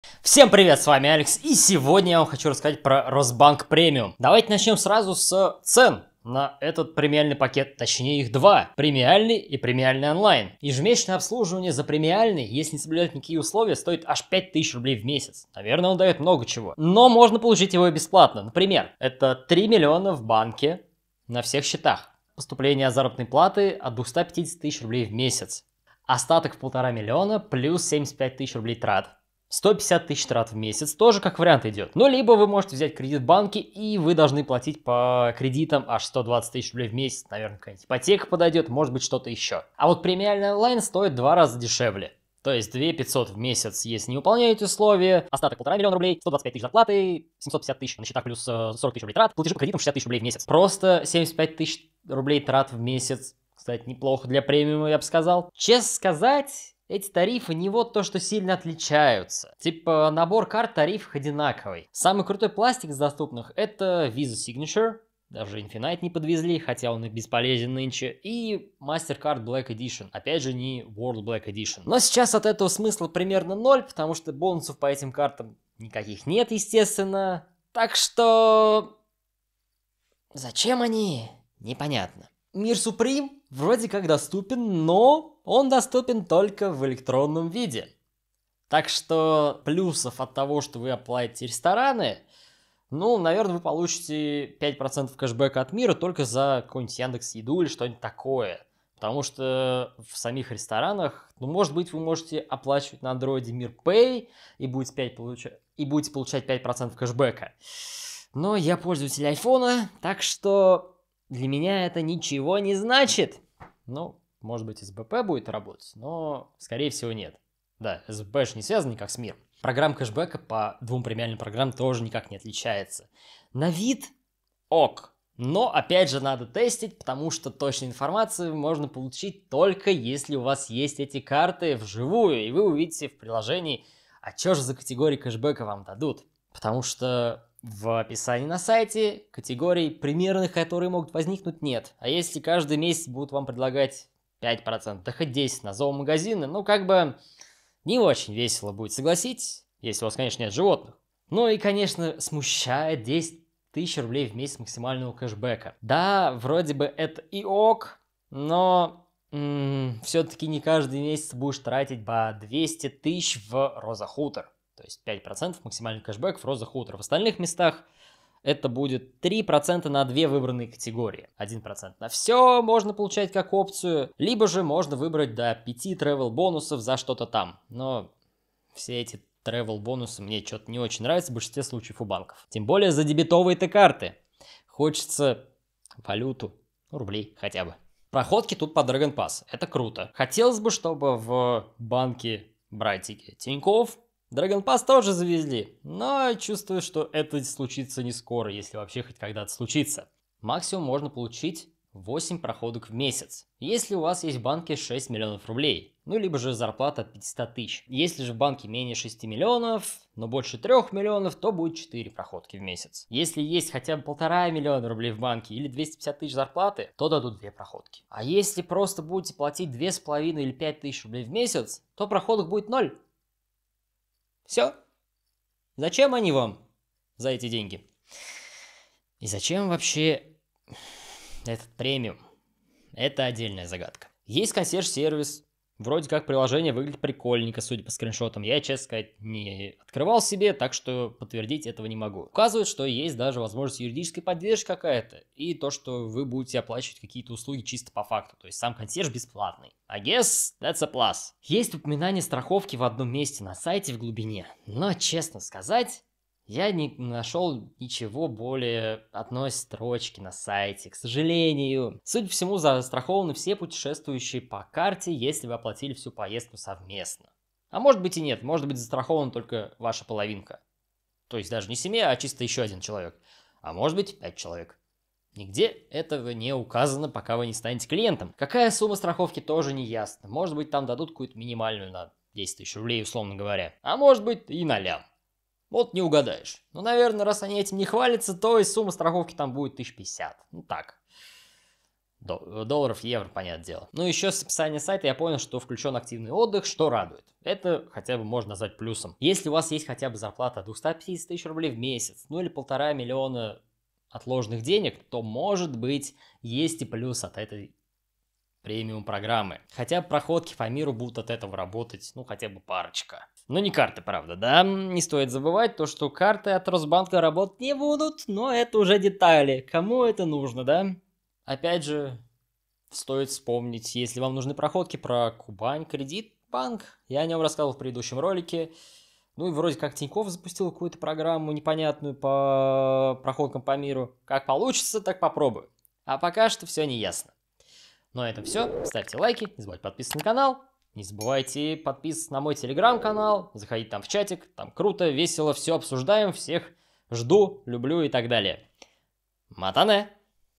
Росбанк Премиум - а зачем их два?
Решил посмотреть в этот раз на Росбанк и их премиум, банк-брат Тинькофф, как никак!